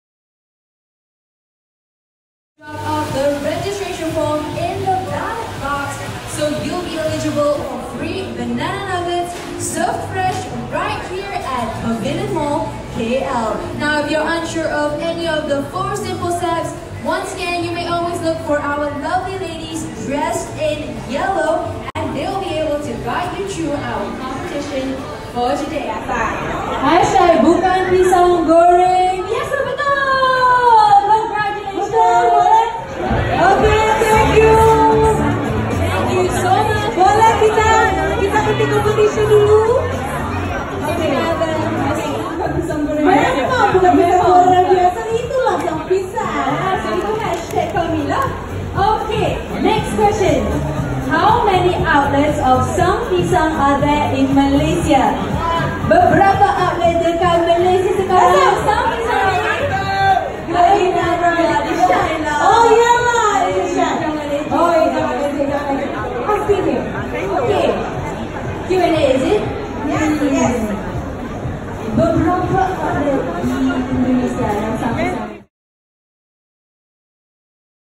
Sang Pisang Grand Launching 2019
Event Host of Sang Pisang's grand launching, the first ever outlet in Malaysia. Officiated by VVIP & VIP guests, Mr Kaesang Pangarep, Mr Faizal and Mr Naim, at Pavilion Mall, on 26th October 2019.